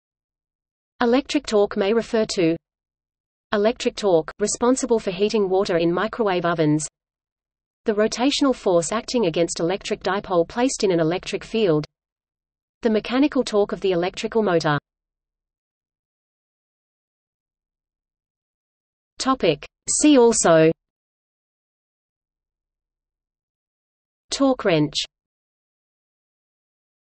Electric torque (disambiguation) | Wikipedia audio article
00:00:15 See also

- Socrates

SUMMARY
Electric torque may refer to:

Electric torque, responsible for heating water in microwave ovens
The rotational force acting against electric dipole placed in an electric field
The mechanical torque of the electrical motor